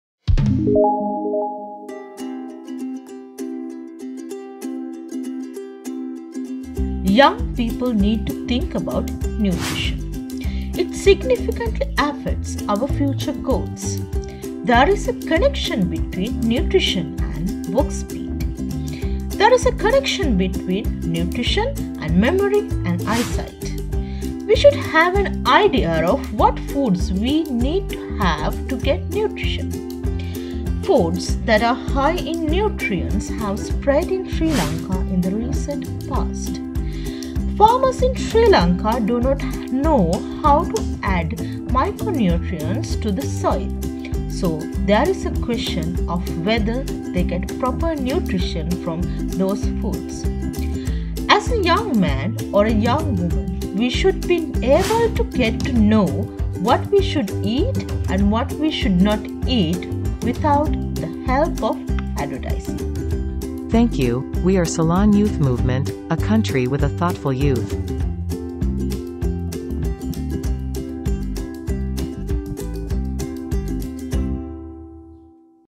Why We Should think about Nutrition _ Ceylon Youth Movement
Young people need to think about nutrition. It significantly affects our future goals. There is a connection between nutrition and work speed. There is a connection between nutrition and memory and eyesight. We should have an idea of ​​what foods we need to have to get nutrition. Foods that are high in nutrients have spread in Sri Lanka in the recent past. Farmers in Sri Lanka do not know how to add micronutrients to the soil, so there is a question of whether they get proper nutrition from those foods. As a young man or a young woman, we should be able to get to know what we should eat and what we should not eat without the help of advertising.